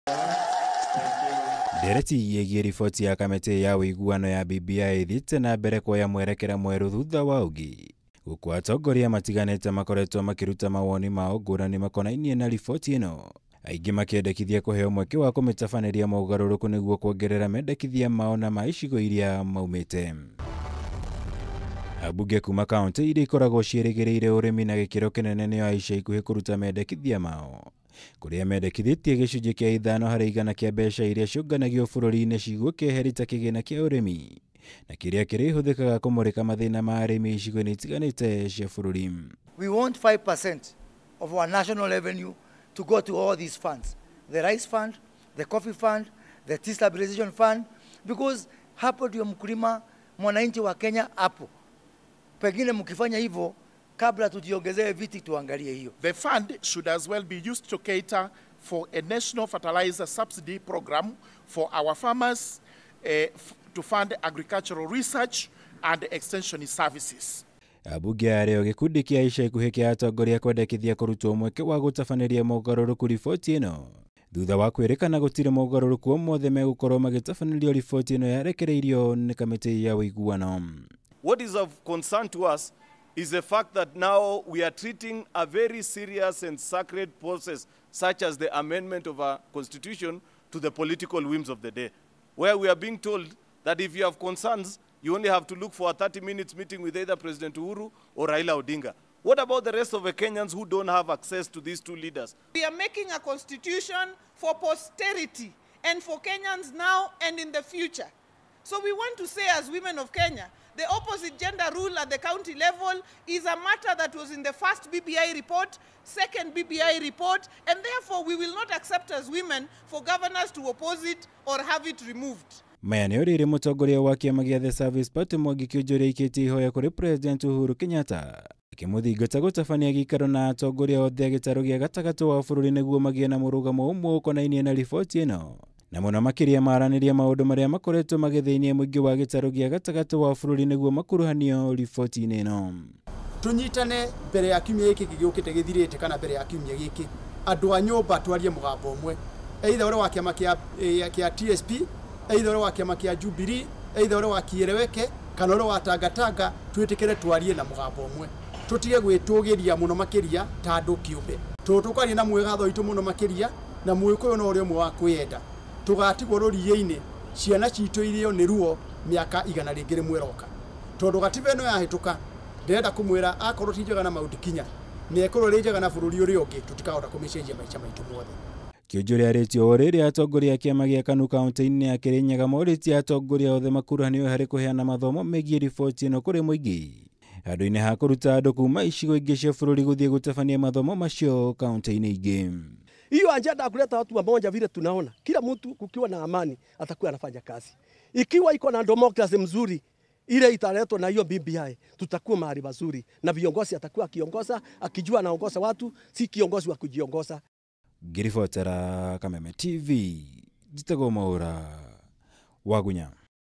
Atongoria kwendekithia maheo mweke gutabania mogaruruku thiini wa BBI
Ambunge kuuma icigo iria ikoragwo irorete iguru urimi nimendekithitie gicunji kia 5 hari igana gia kigina kiria bururi urogotaga kiigwo keheri kurumbuiya mabataro ma arimi, kuhitukira kuumbwo gwa kigina kia urimi na kiria kirihuthikaga kumurika mathina matiganite maria magumagira arimi.
Maritii uu o riri atongoria matiganite mathiite nambere guthing’iriria gukorwo na mweke wa gutabaniria mogaruruku riboti ya kamiti ya BBI, niguo gukuruhania mawoni ma akenya othe bururi handu waruma.